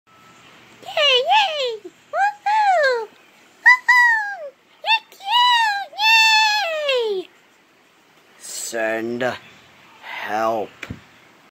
Penglin annoys Bowser
Just a short video of Penglin irritating Bowser. Help this man.